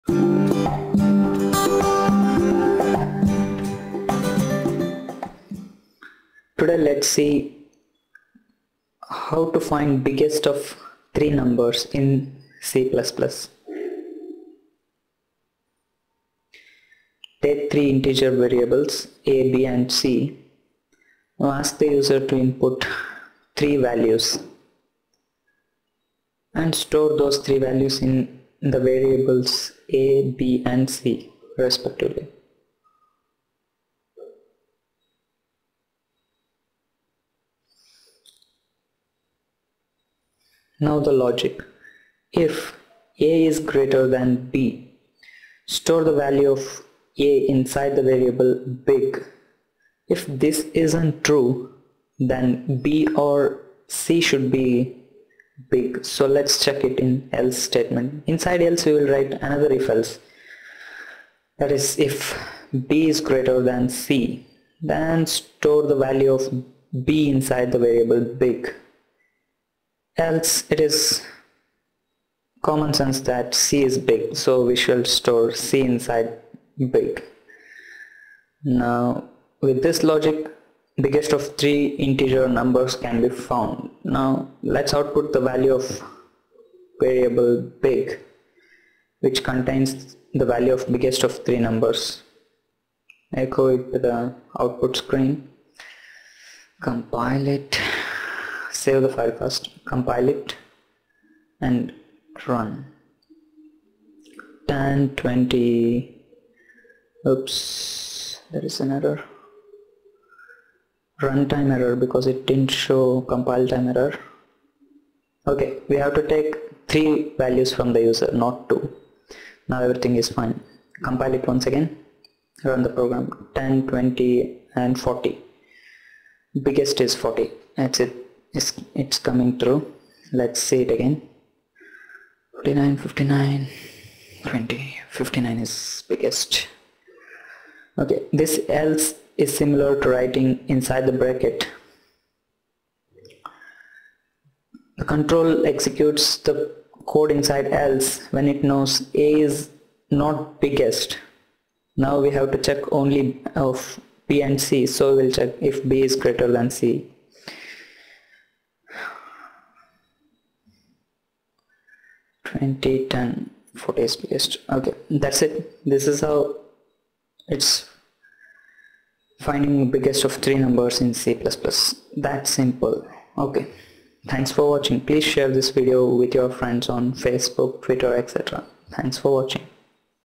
Biggest of 3 Numbers using if-else construct: C++
If value of variable a is bigger, then we store it inside the variable big; else we compare b with c. Now will store the biggest number inside the variable big.